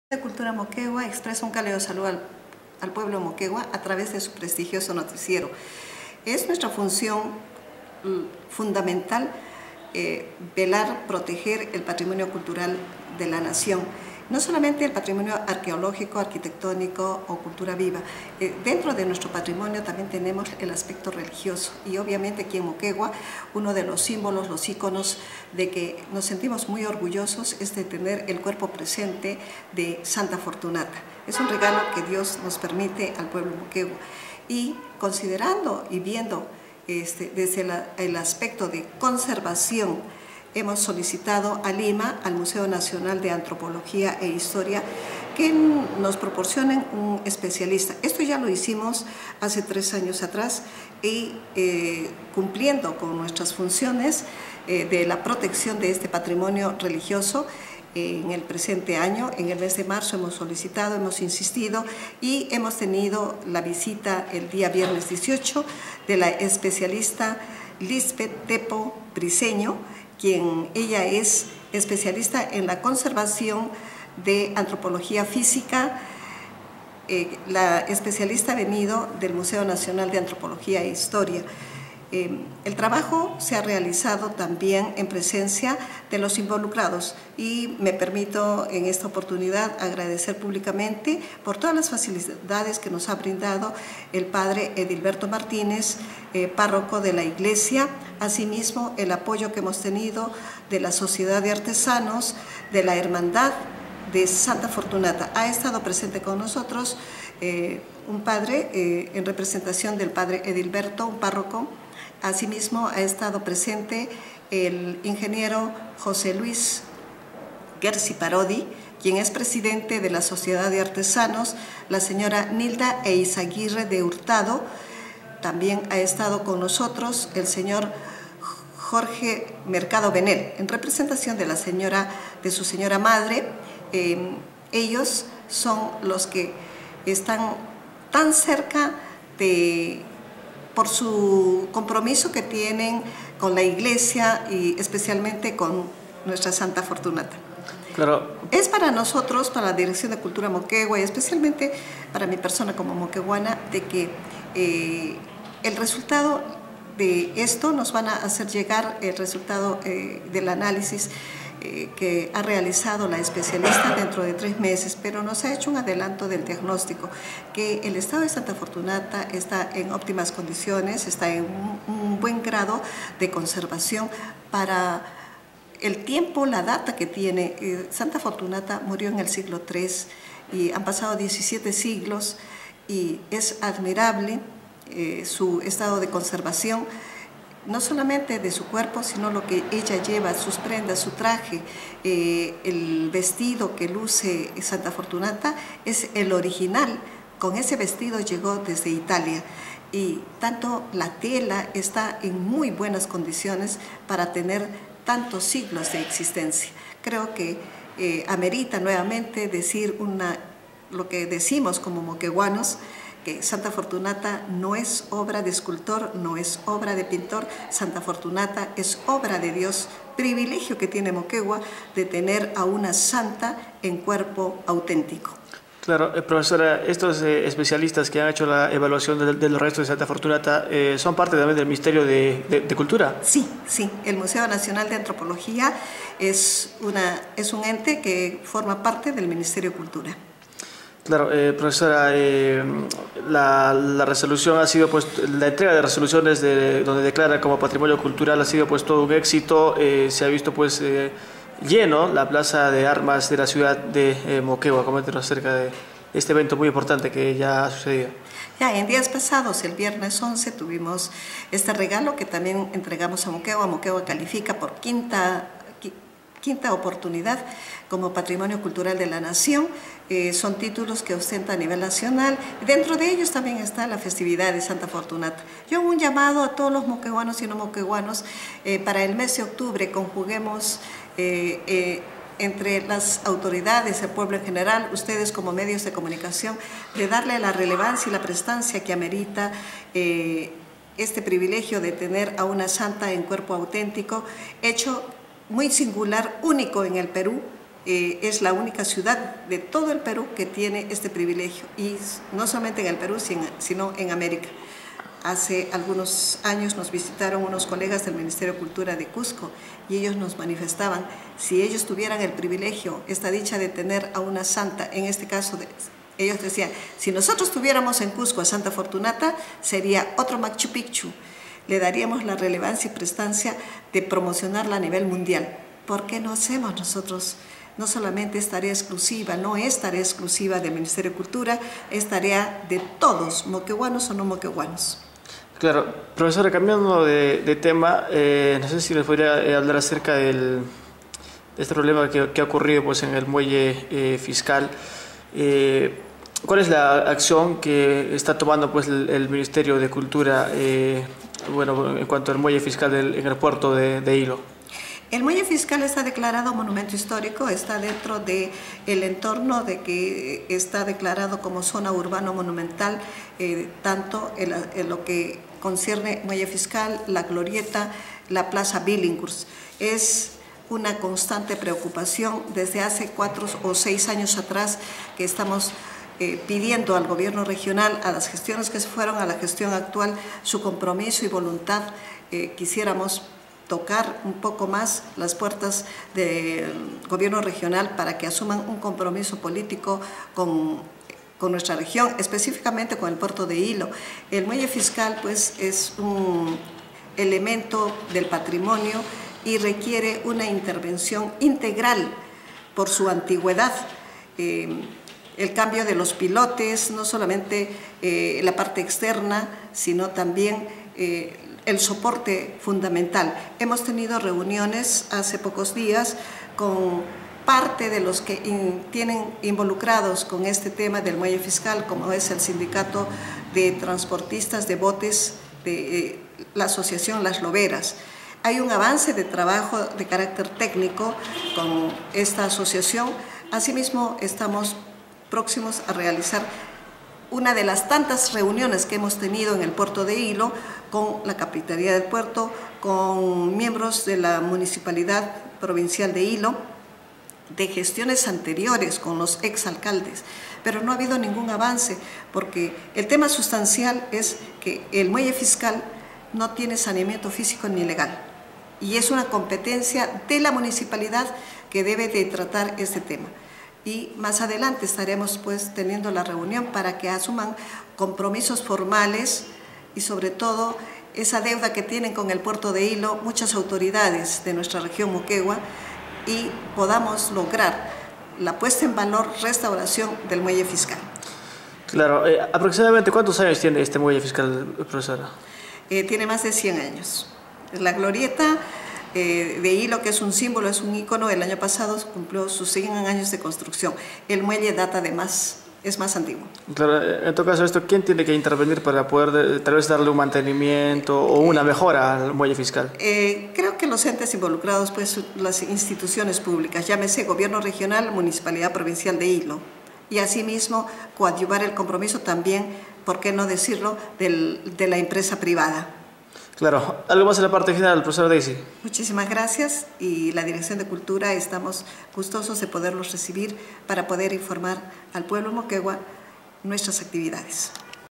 DIRECTORA DE LA OFICINA DESCONCENTRADA DE CULTURA MOQUEGUA.22/08/17
La Profesora Deysi Rivadeneira, Directora de la oficina desconcentrada de cultura Moquegua, manifestó que con especialistas del Ministerio de Cultura, evaluaron restos de Santa Fortunata que se encuentra ubicada dentro de la Cocatedral Santo Domingo.
Rivadeneira indicó que según la evaluación, los restos de Santa Fortunata se encuentran en óptimas condiciones.